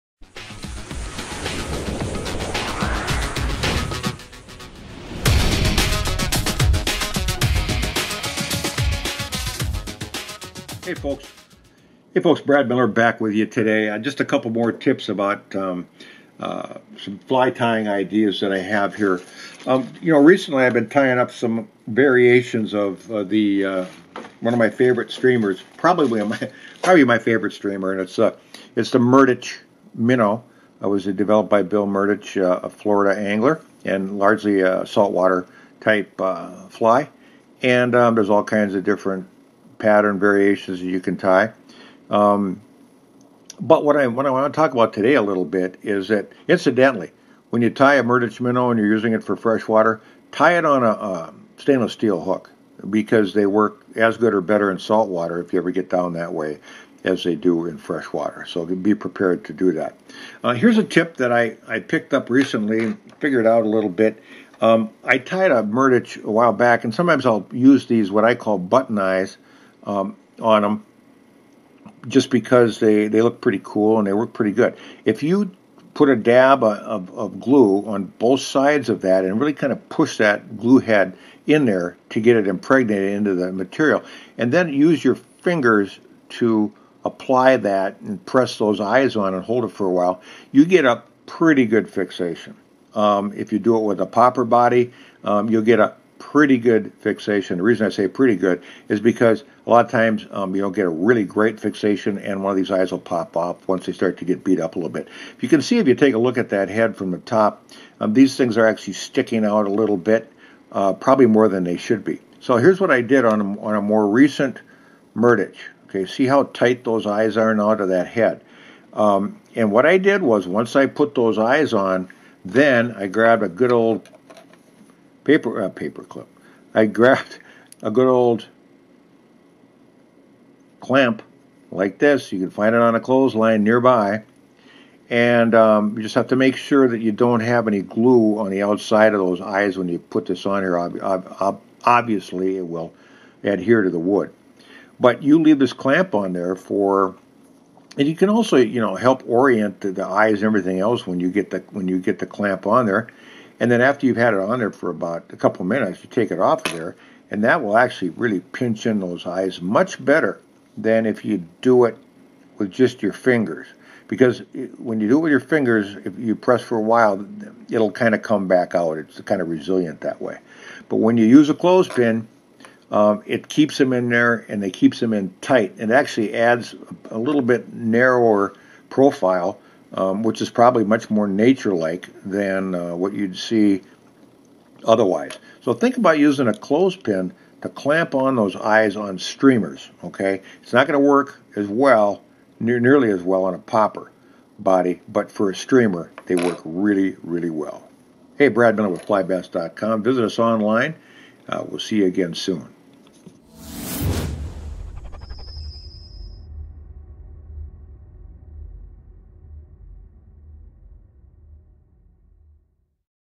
Fly Tying - Setting Streamer Eyes
Brad Miller from FlyBass.com shows a cool technique for glueing streamer eyes in tight to the material using a clothes-pin as a clamp.